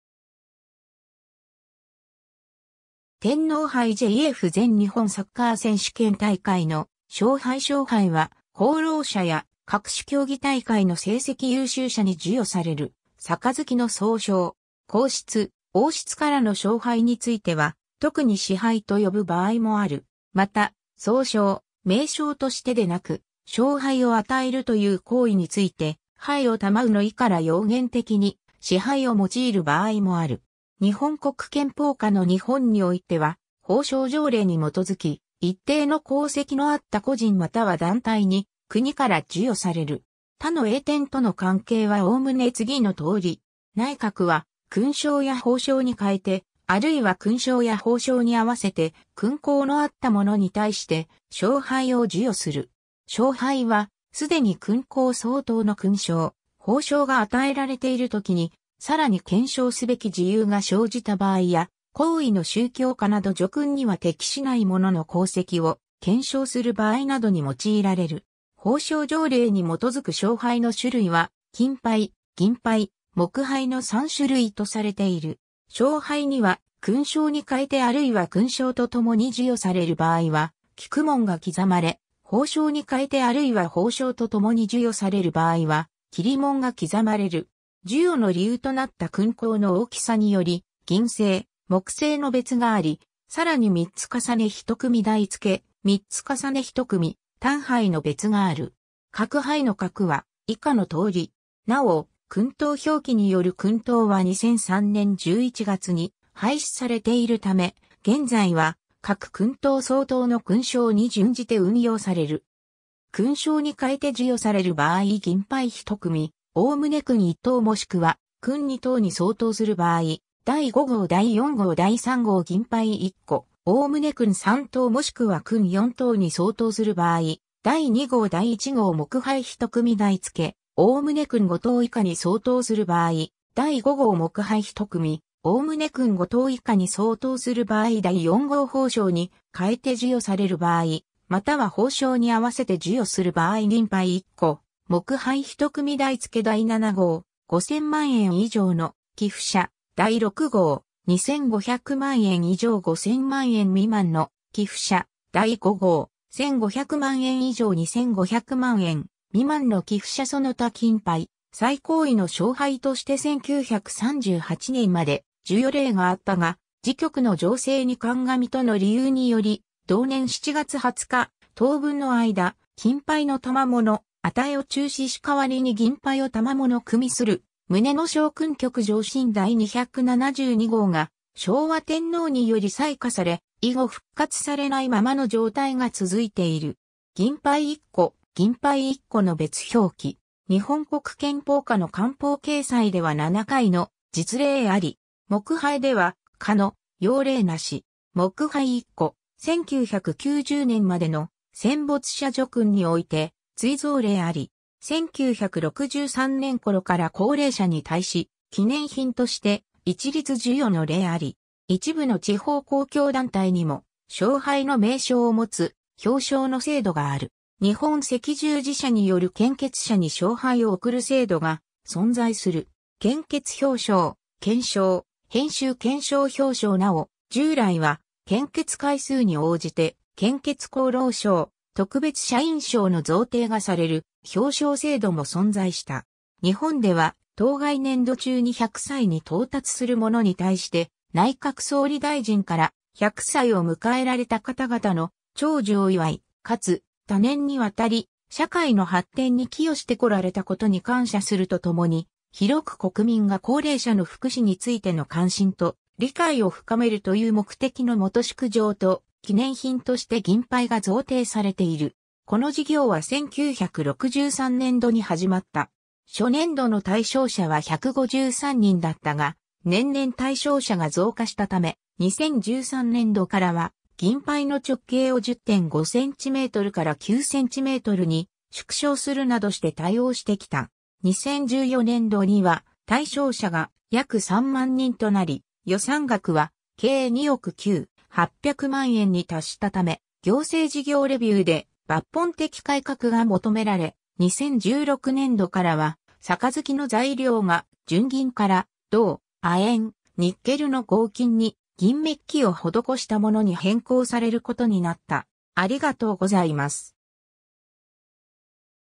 賞杯
天皇杯 JFA 全日本サッカー選手権大会の賞杯（天皇杯） 賞杯（しょうはい）は、功労者や各種競技大会の成績優秀者（個人または団体）に授与される杯の総称。
 皇室・王室からの賞杯については、特に賜杯（賜盃）と呼ぶ場合もある（例:大相撲本場所における(天皇)賜盃）。
また、総称・名称としてでなく賞杯を与えるという行為について「杯を賜う」の意から用言的に「賜杯」を用いる場合もある（例:名称としては「勲章と賞杯」、行為としては「叙勲と賜杯」）。
 日本国憲法下の日本においては、褒章条例（明治14年太政官布告第63号）に基づき、一定の功績のあった個人（遺族）又は団体に国から授与される。
他の栄典との関係はおおむね次のとおり。
 内閣は、勲章や褒章に代えて、あるいは勲章や褒章に併せて、勲功のあった者に対して賞杯を授与する。
賞杯は、既に勲功相当の勲章・褒章が与えられているときにさらに顕彰すべき事由が生じた場合や、高位の宗教家など叙勲には適しない者の功績を顕彰する場合などに用いられる。
 褒章条例に基づく賞杯の種類は、金杯・銀杯・木杯の3種類とされている。
 賞杯には、勲章に代えてあるいは勲章と共に授与される場合は菊紋（菊花紋章）が刻まれ、褒章に代えてあるいは褒章と共に授与される場合は桐紋（五七桐花紋）が刻まれる。
授与の理由となった勲功の大きさにより、銀製、木製（朱塗り）の別があり、さらに三つ重ね一組台付、三つ重ね一組、単杯（一個）の別がある。
各杯の格は、以下のとおり。
なお、「勲○等」表記による勲等は2003年（平成15年）11月に廃止されているため、現在は各勲等相当の勲章に準じて運用される。
 勲章に代えて授与される場合（菊紋付き） 銀杯一組（三つ重ね）： 概ね勲一等若しくは勲二等に相当する場合 第5号（6寸～5寸の杯の三つ重ね） 第4号（5.5寸～4.5寸の杯の三つ重ね） 第3号（5寸～4寸の杯の三つ重ね） 銀杯一個： 概ね勲三等若しくは勲四等に相当する場合 第2号（5.4寸） 第1号（4.2寸） 木杯一組台付： 概ね勲五等以下に相当する場合 第5号（3.6寸～2.7寸の杯の三つ重ね） 木杯一組： 概ね勲五等以下に相当する場合 第4号（4.6寸～3.4寸の杯の三つ重ね） 褒章に代えて授与される場合または褒章に併せて授与する場合（桐紋付き） 銀杯一個（4.2寸） 木杯一組台付 第7号（4.6寸～3.4寸の杯の三つ重ね）： 5000万円以上の寄付者（紺綬褒章に併賜） 第6号（4.2寸～3寸の杯の三つ重ね）： 2500万円以上5000万円未満の寄付者（紺綬褒章に併賜） 第5号（3.6寸～2.7寸の杯の三つ重ね）： 1500万円以上2500万円未満の寄付者（紺綬褒章に併賜） その他 金杯： 最高位の賞杯として1938年（昭和13年）まで授与例があったが、「時局の情勢に鑑み」との理由により、同年7月20日「当分の間、金杯の賜与を中止し代わりに銀杯を賜与する」旨の賞勲局上申第272号（褒章条例ニ依ル金杯ノ賜与等ニ関スル件）が昭和天皇により裁可され、以後復活されないままの状態が続いている。
 銀杯一箇： 銀杯一個の別表記。
日本国憲法下の官報掲載では7回（計24人）の実例あり。
木杯では「箇」の用例なし。
 木杯一個： 1990年までの戦没者叙勲において追贈例あり。
1963年頃から高齢者（100歳又は88歳）に対し記念品として一律授与の例あり。
 一部の地方公共団体にも、賞杯の名称を持つ表彰の制度がある。
 日本赤十字社による献血者に賞杯を贈る制度が存在する。
 献血表彰・顕彰（賞杯）[編集] 顕彰（賞杯） 表彰 なお、従来は献血回数に応じて献血功労章（ピンバッジ）、特別社員章の贈呈がされる表彰制度も存在した。
 日本では当該年度中に100歳に到達する者（海外在留邦人及び永住している在日外国人を含む）に対して内閣総理大臣から「百歳を迎えられた方々の長寿を祝い、かつ、多年にわたり社会の発展に寄与してこられたことに感謝するとともに、ひろく国民が高齢者の福祉についての関心と理解を深める」という目的のもと祝状と記念品として銀杯が贈呈されている。
 この事業は1963年度（昭和38年度）に始まった。
初年度の対象者は153人だったが、年々対象者が増加したため、2013年度（平成21年度）からは銀杯の直径を10.5cmから9cmに縮小するなどして対応してきた。
2014年度（平成26年度）には対象者が約3万人となり予算額は計2億9,800万円に達したため、行政事業レビューで抜本的改革が求めら...